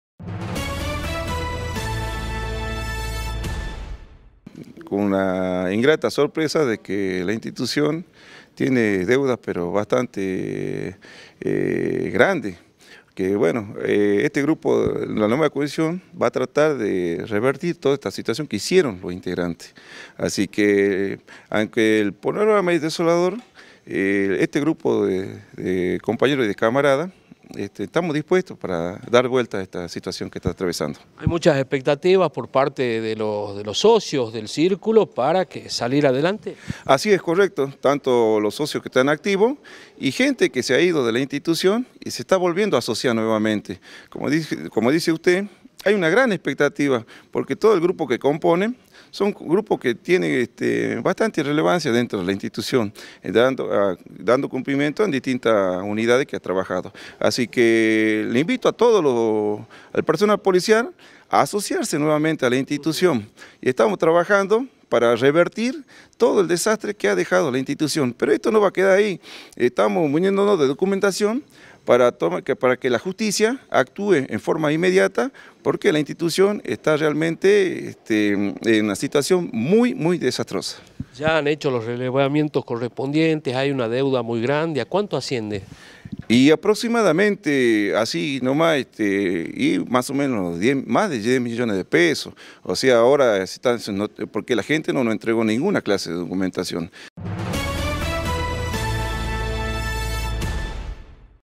Deuda millonaria del Círculo Policial
La institución está debiendo más de diez millones de pesos, así lo dijo el Secretario General, Julio Borja. Asumió la nueva comisión directiva.